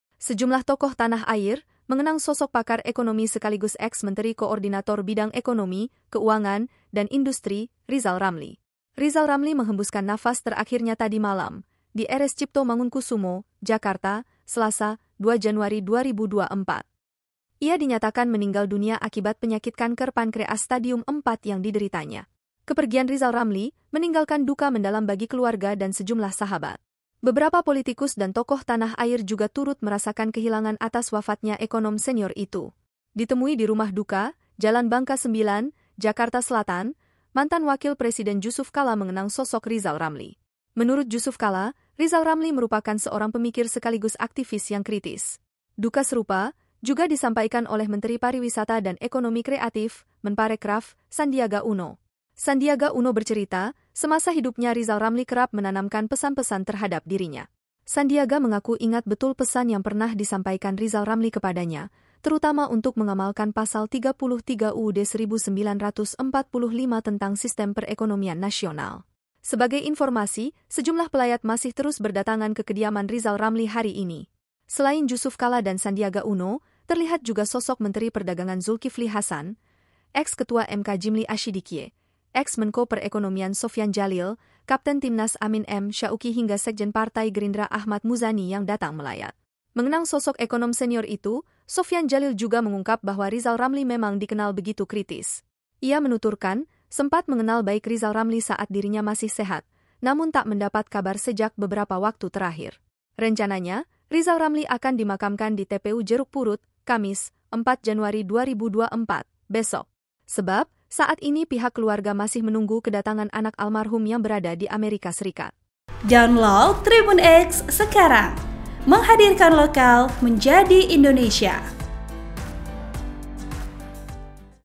Sejumlah Tokoh Tanah Air Kenang Sosok Rizal Ramli, Jusuf Kalla: Beliau Seorang Pemikir dan Aktivis
TRIBUN-VIDEO.COM - Sejumlah tokoh Tanah Air, mengenang sosok Pakar Ekonomi sekaligus eks Menteri Koordinator Bidang Ekonomi, Keuangan, dan Industri, Rizal Ramli.

Rizal Ramli menghembuskan nafas terakhirnya tadi malam, di RS Cipto Mangunkusumo, Jakarta, Selasa (2/1/2024).